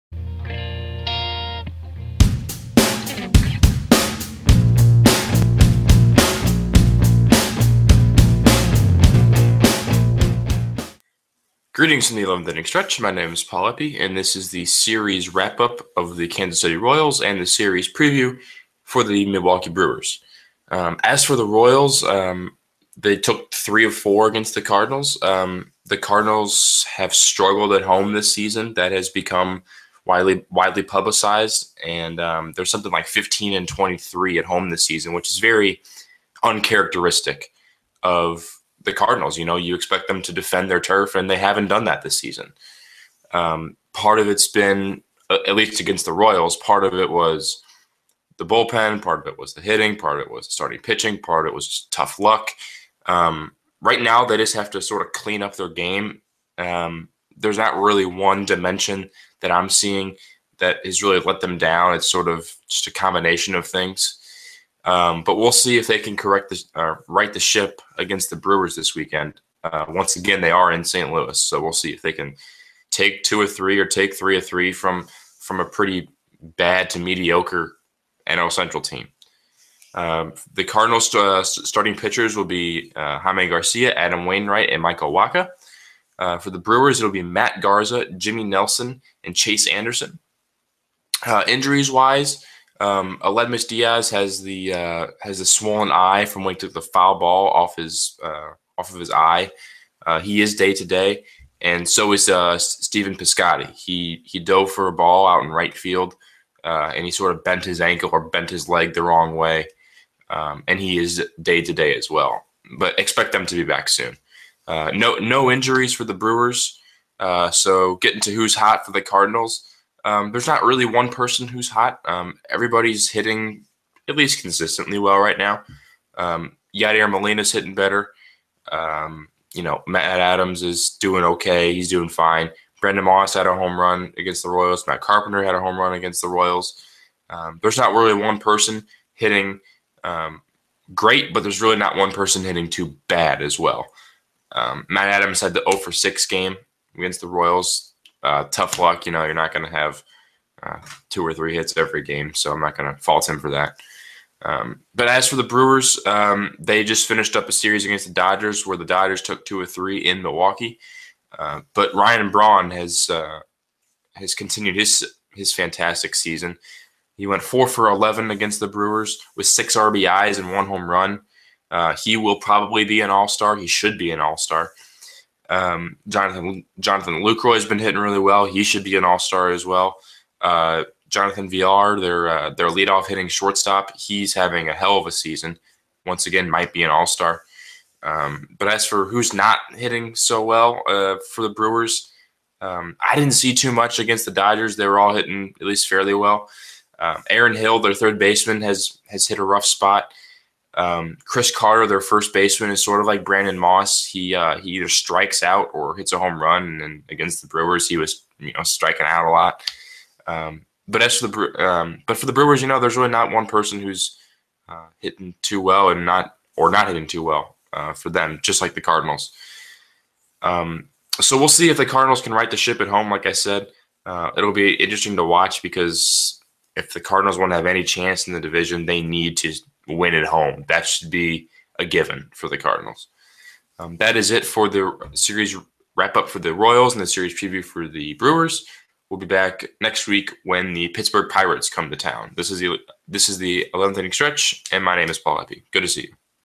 Series 26 - Milwaukee Brewers
The 4th place Brewers visit the 2nd place Cardinals in St. Louis.